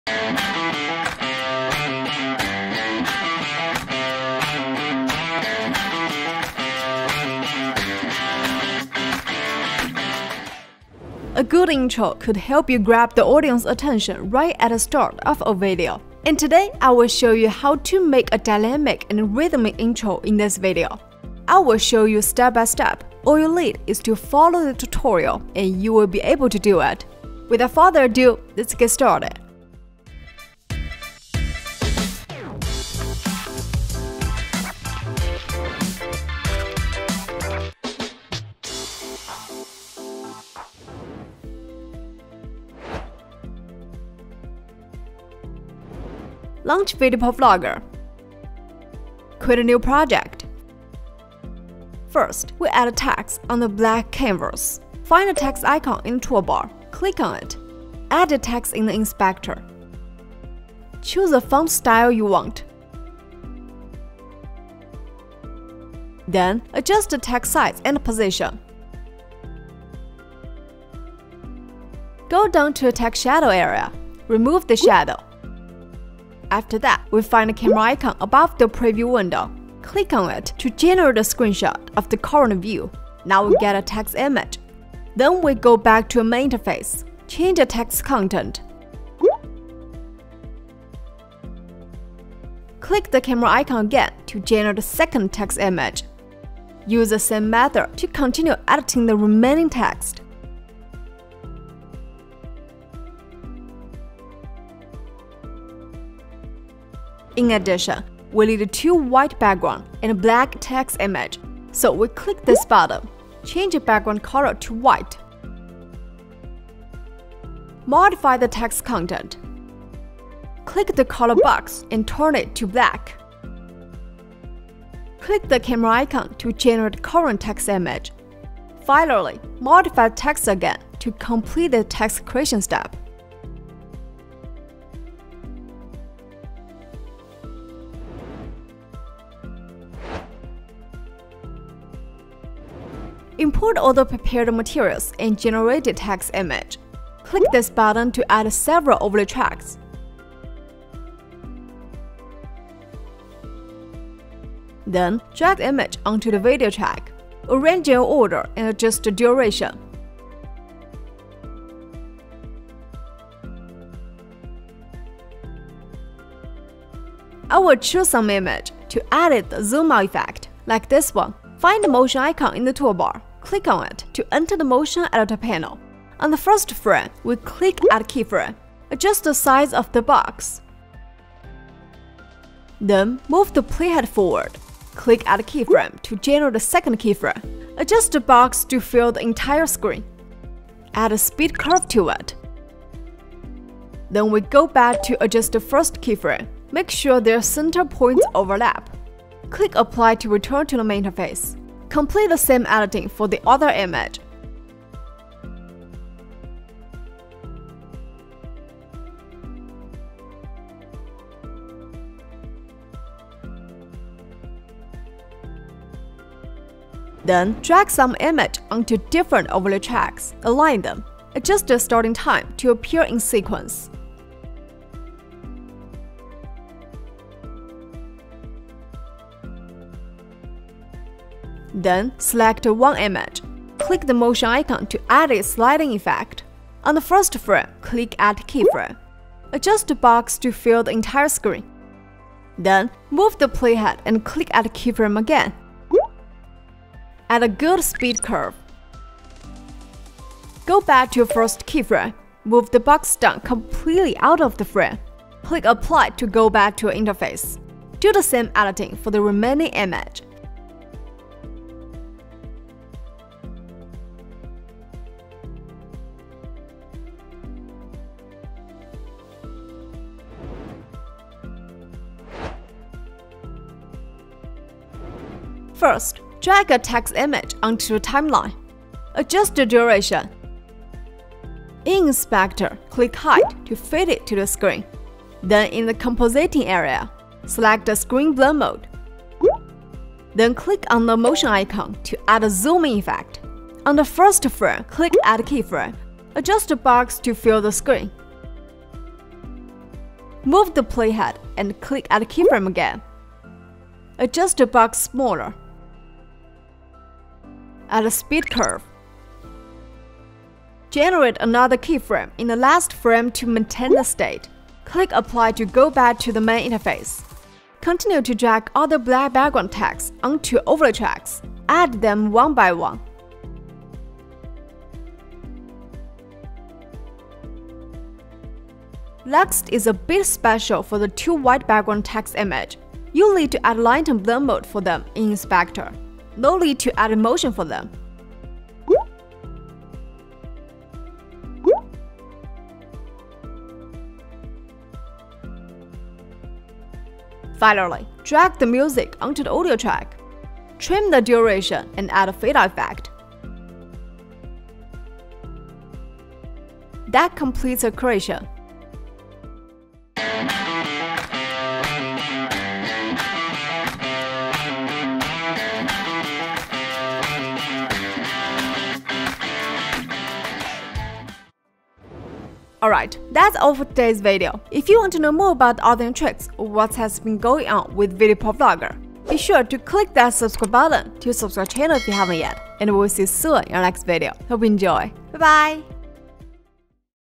How to Create a Dynamic Video Intro - VideoProc Vlogger Tutorial
➜ VideoProc Vlogger FREE

Creating a captivating video intro is crucial for grabbing your audience's attention and setting the tone for your content. In this video, we are going to show you how to create a dynamic stomp intro for your video. If this is what you are looking for, check out the tutorial and learn the easiest way to create a visually engaging and energetic intro for your video!

Timestamp:
00:00 Intro
00:45 Create text
02:19 Edit picture effect
04:33 Edit text effect